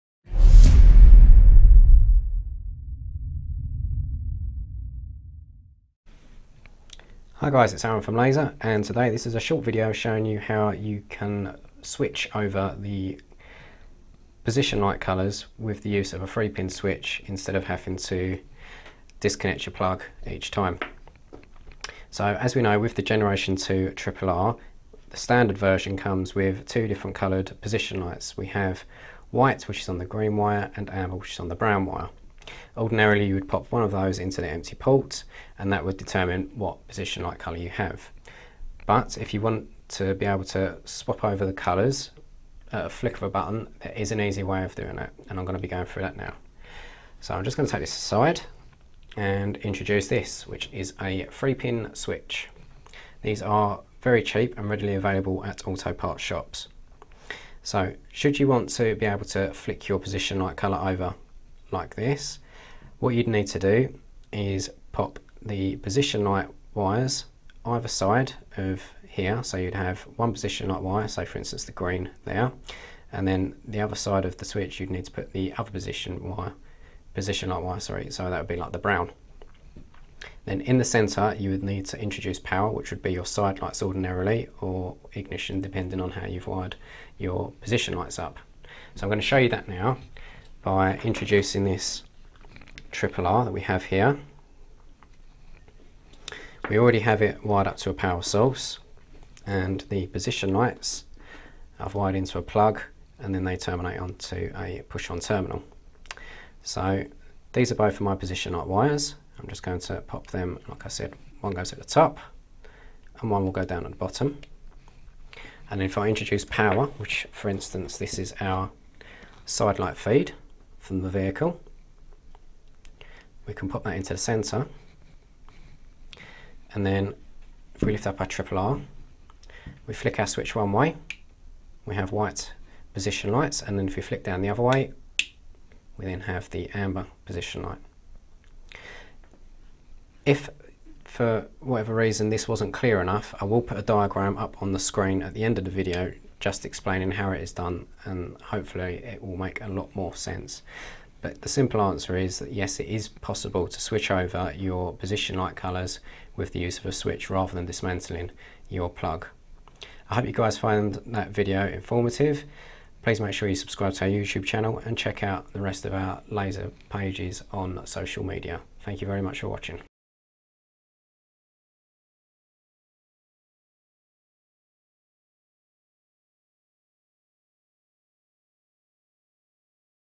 HOW TO: 3-Pin Switch for White and Amber Position Lights
In this short video, we demonstrate how to connect our latest generation of Triple-R products with white and amber position/side light functions, so that at the flick of a switch you can alternate between the different colours, or turn off the position/side light function altogether and just have the lights operating as high beam driving lights only.

This solution requires purchase of a separate 3-pin switch not included with the light or wiring harness kits. The same process applies to other Lazer lamps products with white and amber position light options, including the Sentinel range of round LED lights.